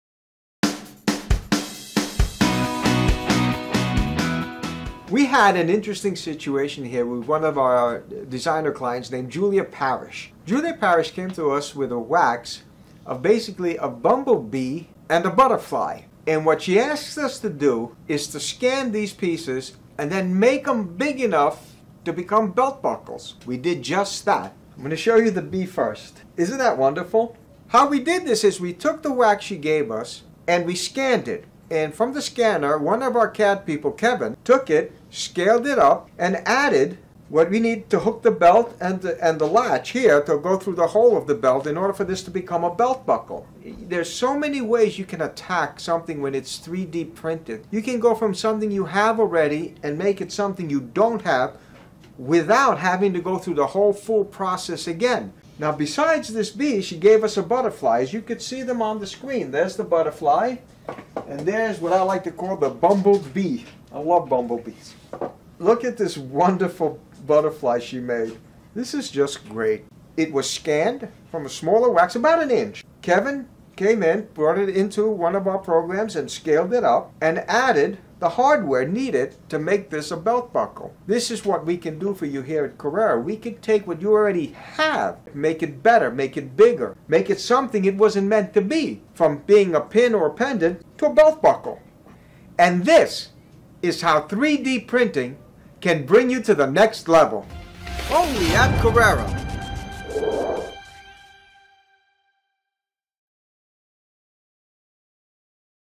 Custom Belt Buckles At Carrera Casting
Julia Parish came to Carrera a while back with a quite unusual project in mind. She had these small eccentric butterfly and bumble bee hand carved waxes she wanted to turn into belt buckles. Carrera's amazing R&D team set out to the task. We scanned, blew up, and refined her designs for a fashion forward belt buckle. Follow along this quick, simple, and affordable journey how you can create an entire collection just from 1 wax!